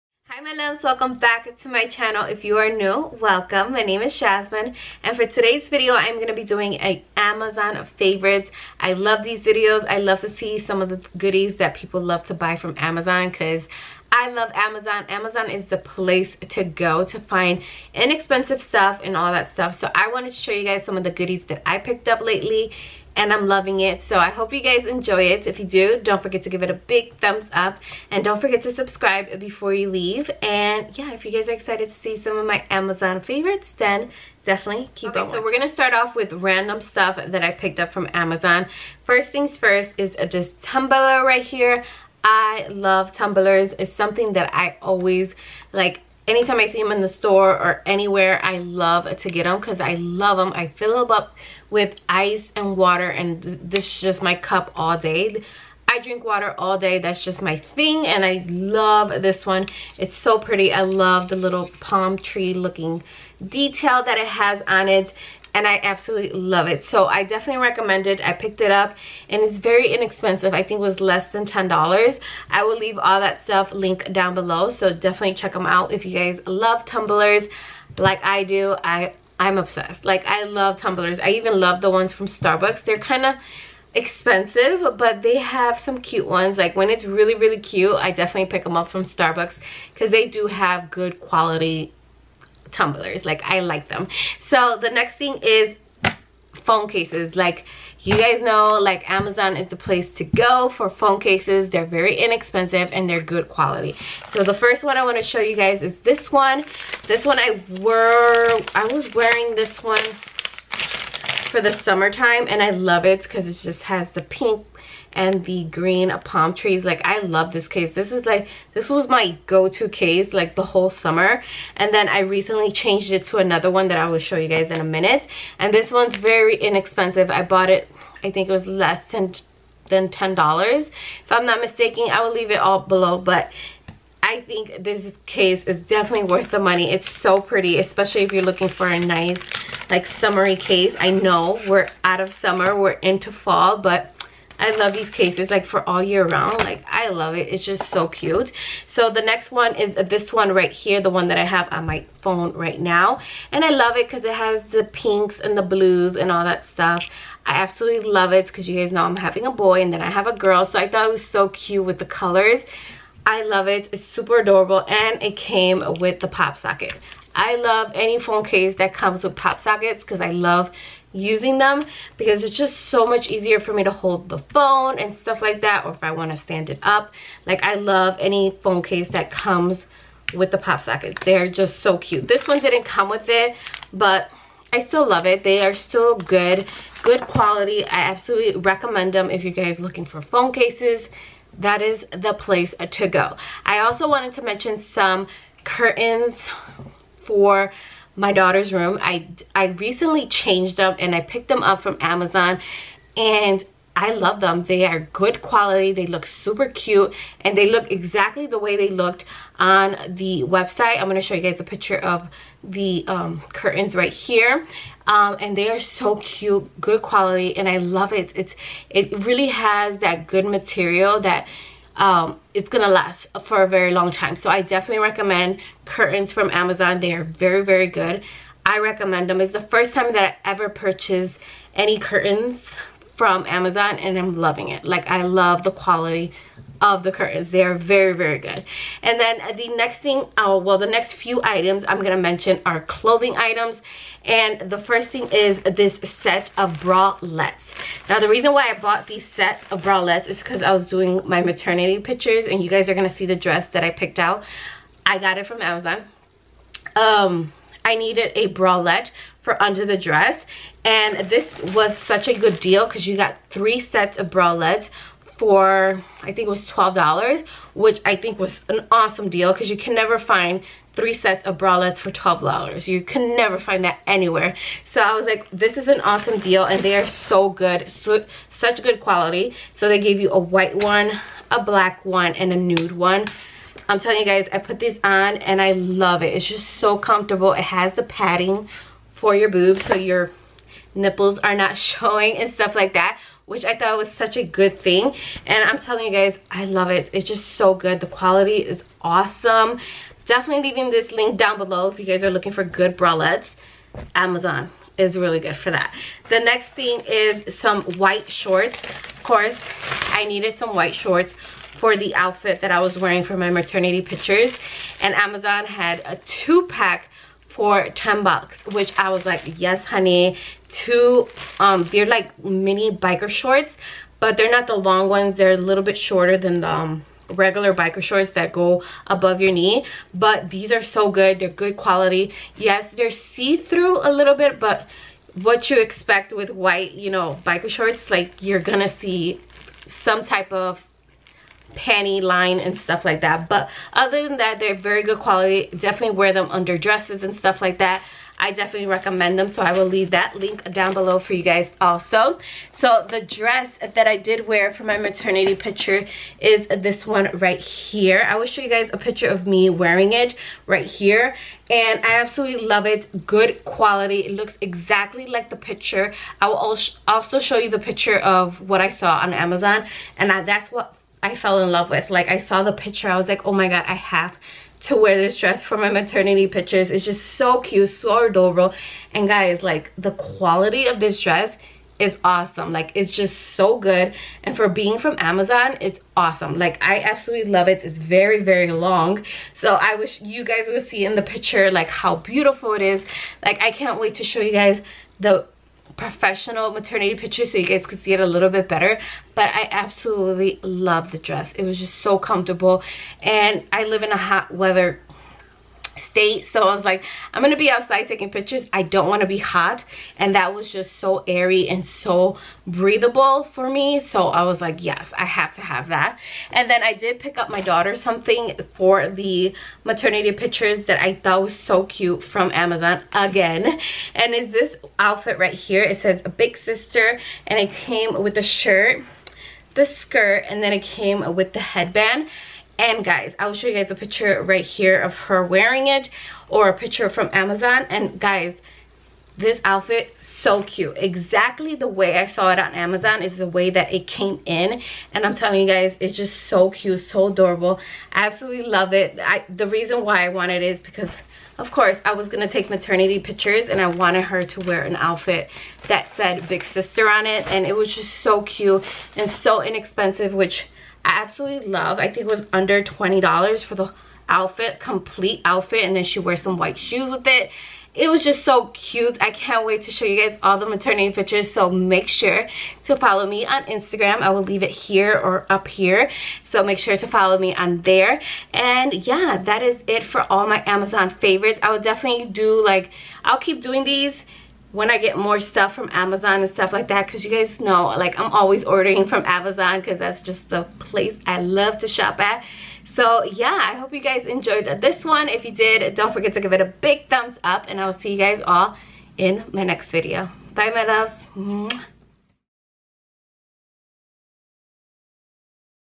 Amazon Favorites | Amazon Fashion Haul 2019
Hi Loves, 💕

Favorites:

Similar tumblr- Palm Leaves Tumbler with Straw and Lid Acrylic Plastic Insulated Double Wall Cup 17.5 oz

Phone cases - Ownest Compatible with iPhone Xs...

Curtains- Utopia Decor Bedroom Curtains...

Bras - Boao 3 Pieces V Neck Tube Top Bra...

Shorts - Gilbins 2 Pack Women's Seamless...

Maternity Dress- Pregnant Women Photography Lace...

Big sister outfit- Big Little Sister Bodysuit Letter...

YouTube library
Free downloads